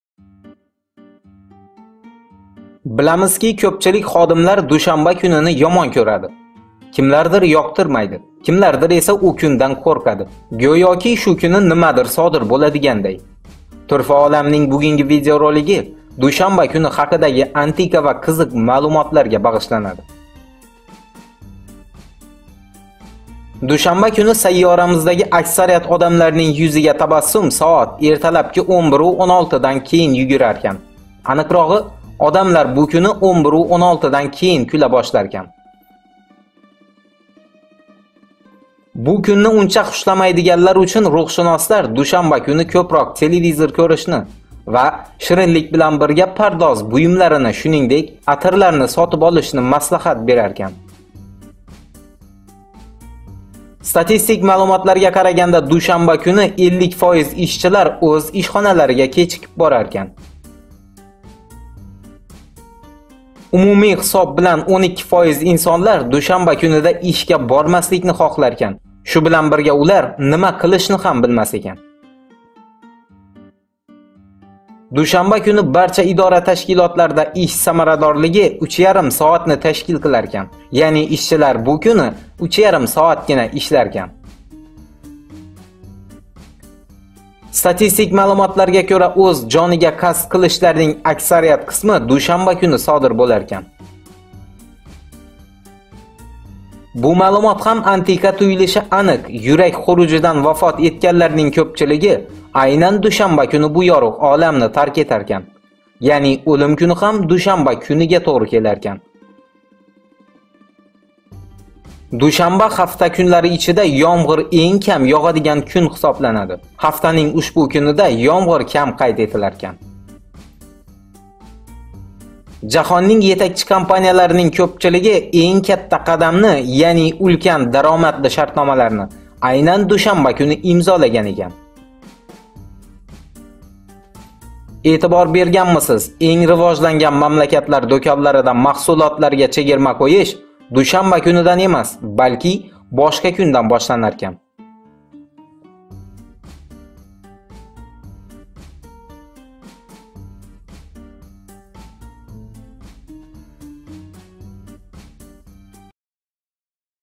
ДУШАНБА КУНИ ХАКИДА ЭНГ КИЗИК 10 ТА ФАКТ
Биламизки купчилик ходимлар душанба кунини емон куради.Кимлардир ектирмайди кимлардир эса
у кундан куркади.Гуеки шу куни нимадир буладигандай.Турфа Оламнинг бугунги видеоролиги душанба куни хакидаги антика маълумотларга багишланади.

Произведение «ДУШАНБА КУНИ ХАКИДА ЭНГ КИЗИК 10 ТА ФАКТ» созданное автором по имени AKATOR, публикуется на условиях лицензии Creative Commons «Attribution» («Атрибуция») 4.0 Всемирная.